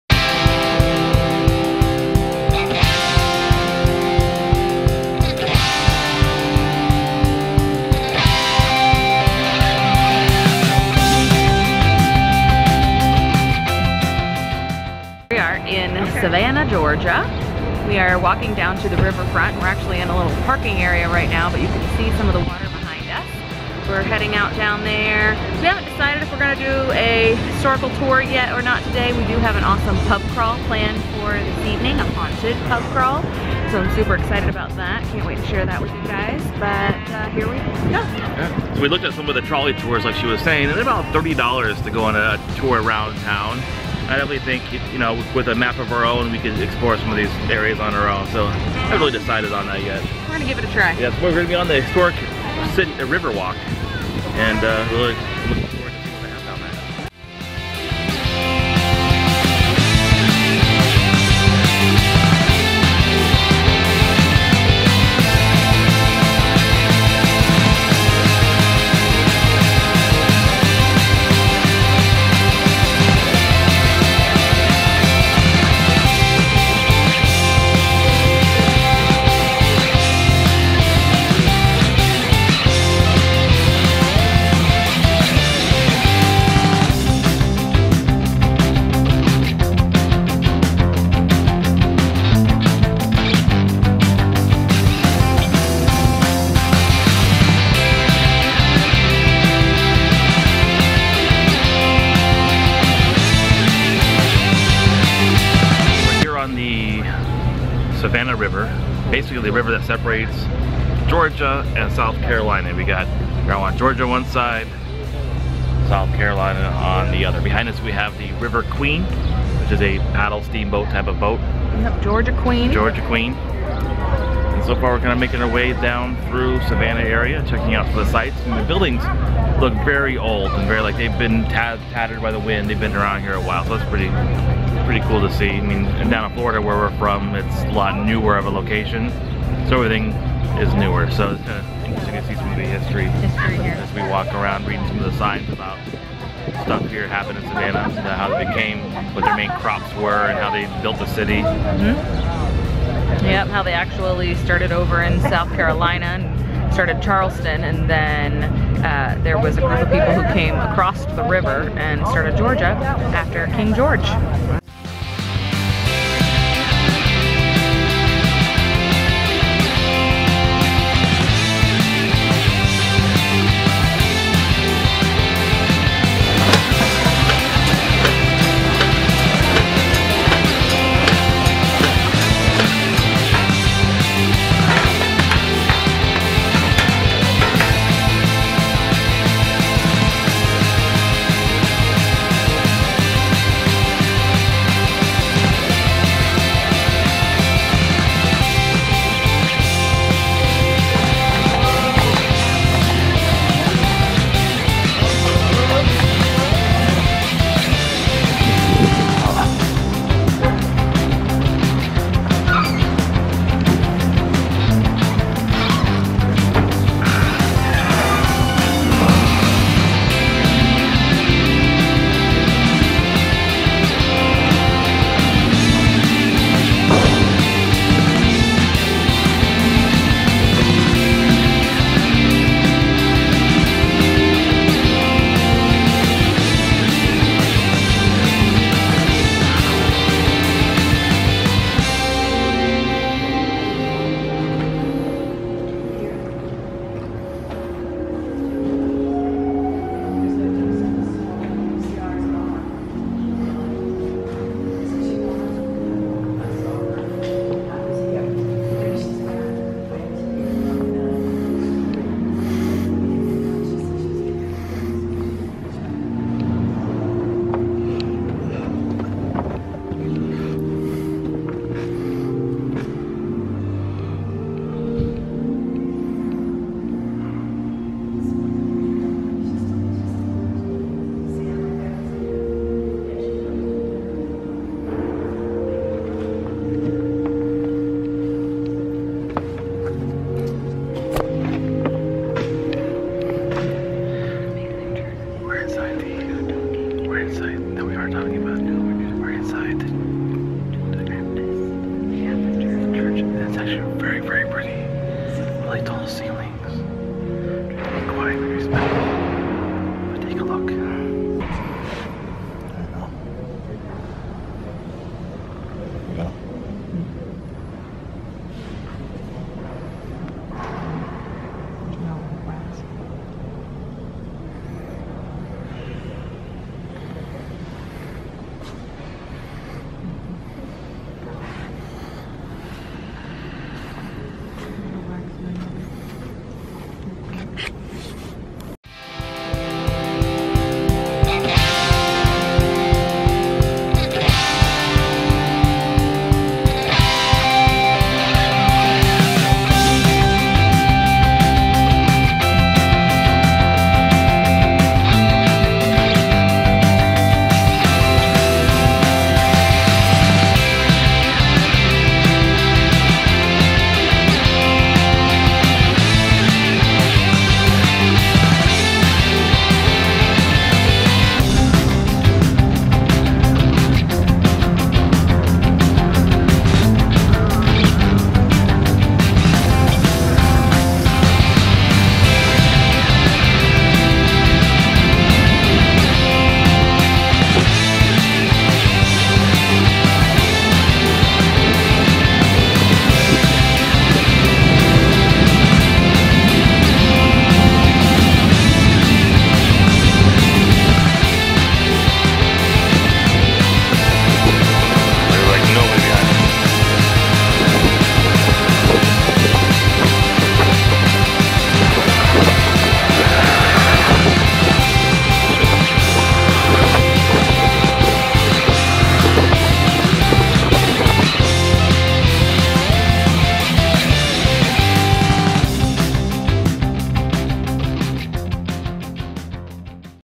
Savannah, Floods, and catholics
We spent the day in Savannah Georgia, exploring this historical city! We were able to step inside the John the Baptist Catholic Church and it was gorgeous!! When we were heading home... it took us 2 hours to get out of the area due to flooding. There was a tropical storm that came through the night before we left.